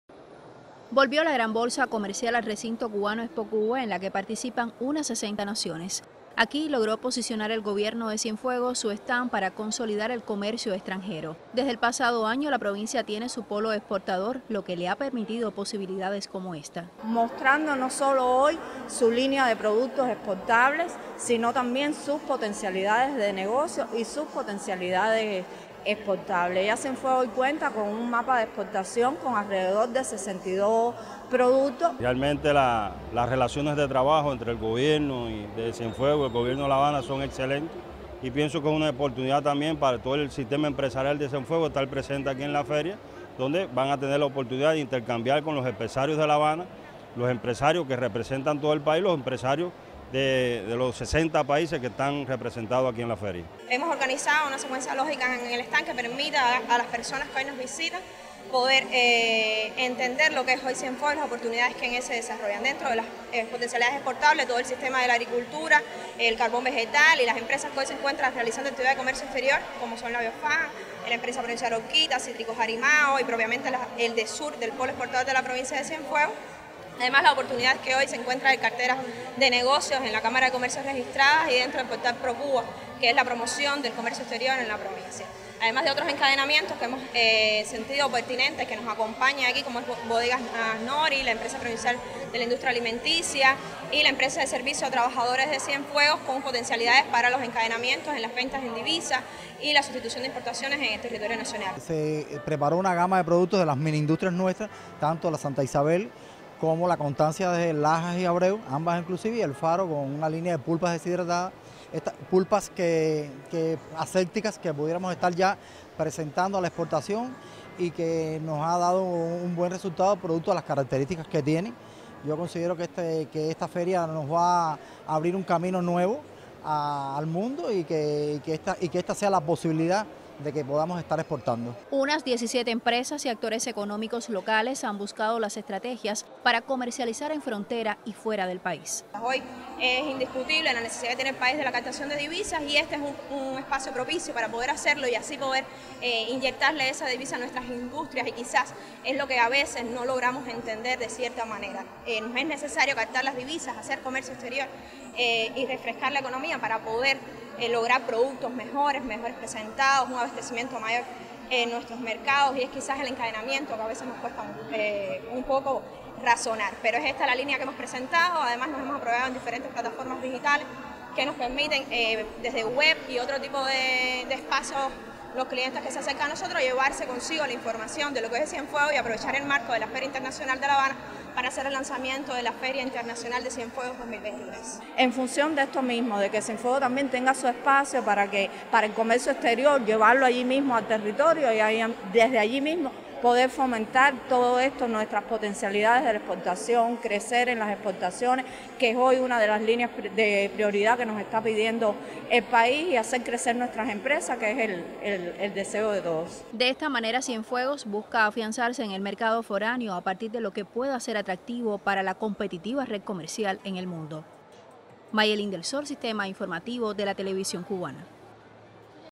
Cienfuegos por vez primera en Feria Internacional de La Habana
Por vez primera los Gobiernos de Cienfuegos y La Habana están representados con stand de oportunidades en la Feria Internacional que por estos días sesiona en PABEXPO. A partir de las propuestas de los departamentos de Comercio Exterior, para ambas provincias representa un momento para establecer negocios con entidades nacionales y extranjeras.